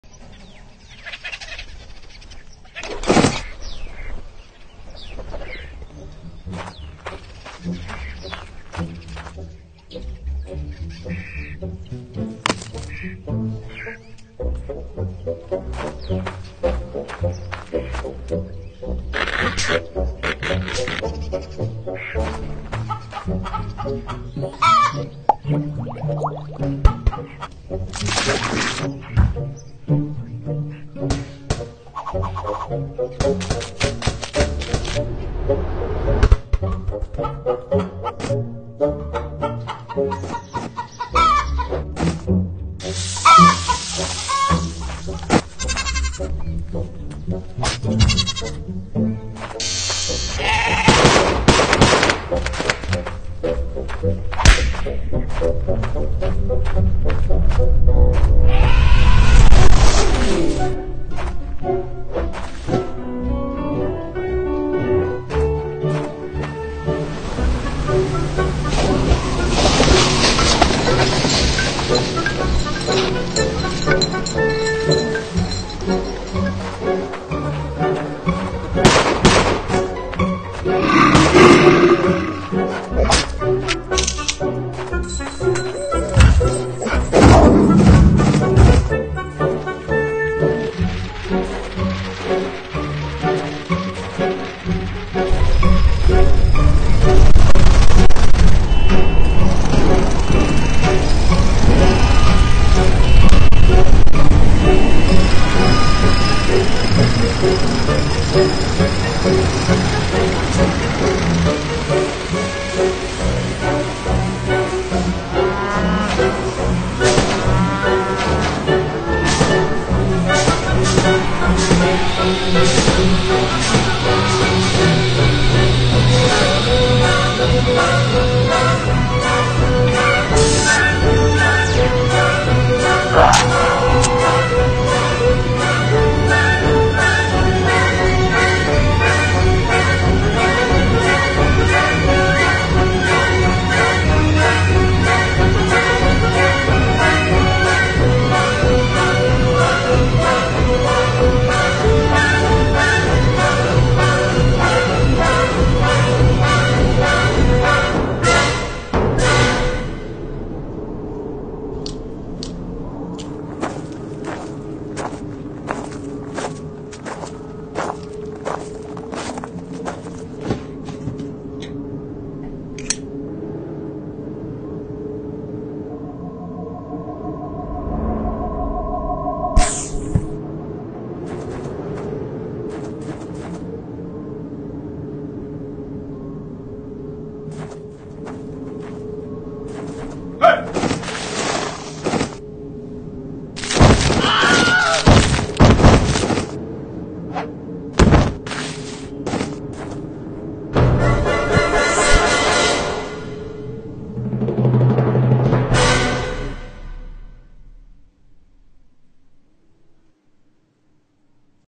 Human on earth
Sad reality on
How men uses earth resource